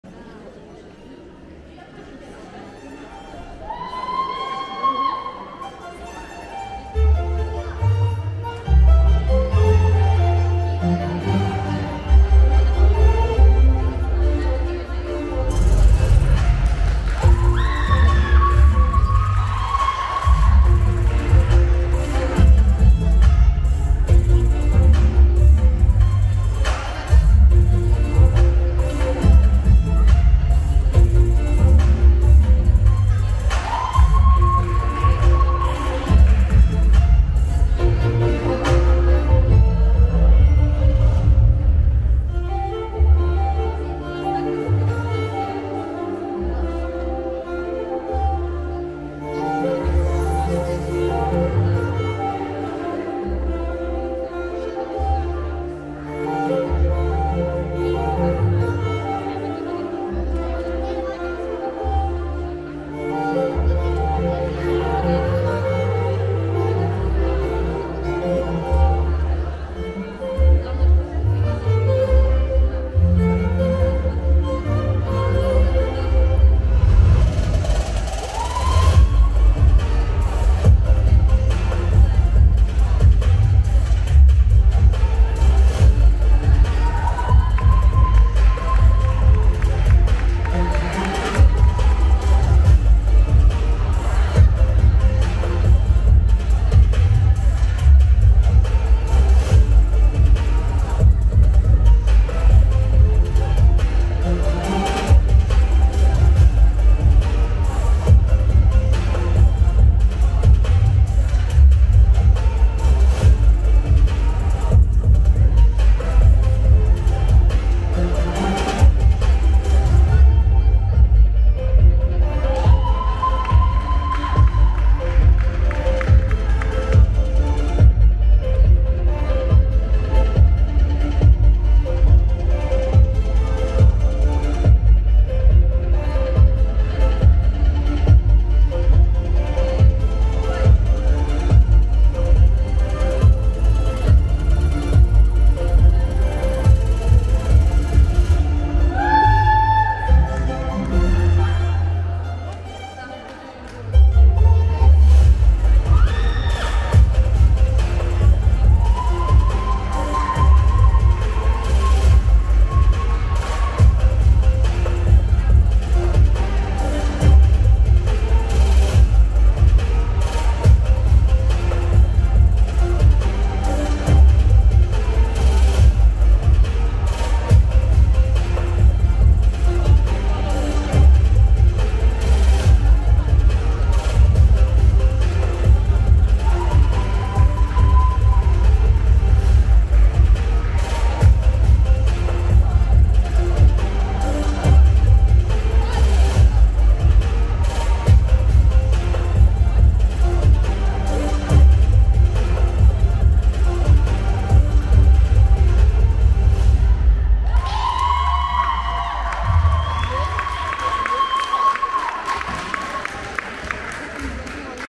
Muratova Rada (Edem, Karaganda, KAZ)
2 место
Спорт на пилоне Pole Sport
11-12 лет Юношеский Возраст (Novice)
Начинающий (Amateur)
Республиканский Турнир Республики Казахстан 2023 по спорту на пилоне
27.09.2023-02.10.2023
город Астана
Astana city, Kazakshatn
Pole Sport Championship
Kazakhstan Republic